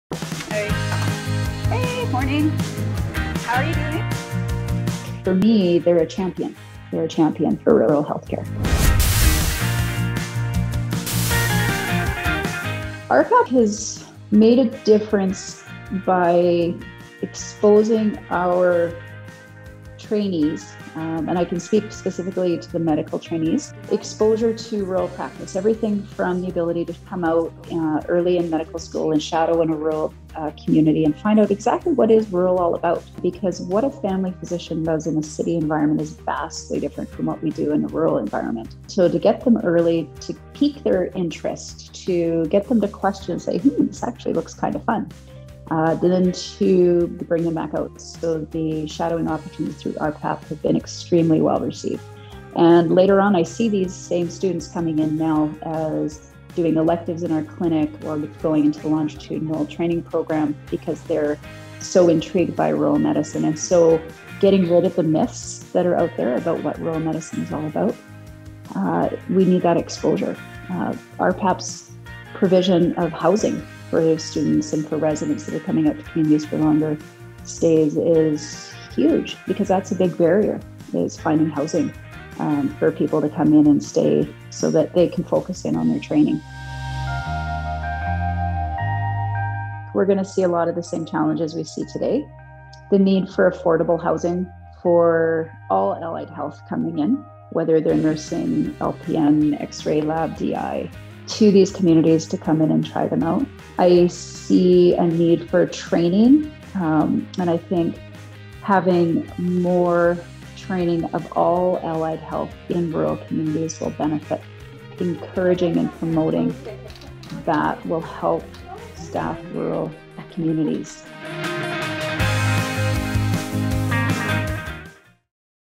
Sundre's Dr. Michelle Warren describes RhPAP as a champion for rural health care
Dr. Michelle Warren appreciates the opportunities RhPAP has provided for medical students and residents to explore rural practice.

Dr. Warren is the 2016 RhPAP Alberta Rural Physician Award of Distinction recipient. She is also President-Elect of the Alberta Medical Association and will step into the President role in late September.

In our 18th RhPAP 30th Anniversary video, Dr. Warren shares what she thinks are the biggest challenges that RhPAP should tackle in the next 30 years.